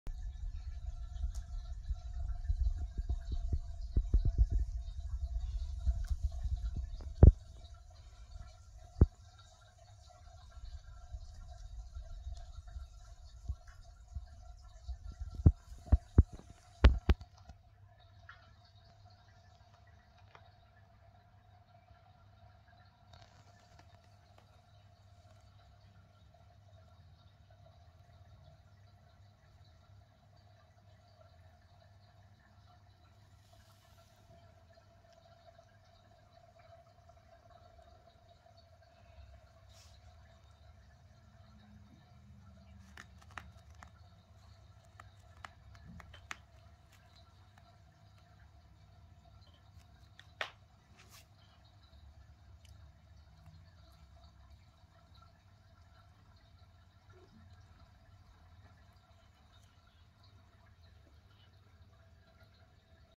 2 cute scottish fold kittens . First time out of her place to play
قطط اسكوتش فولد. أول مرة تخرج من مكانها للعب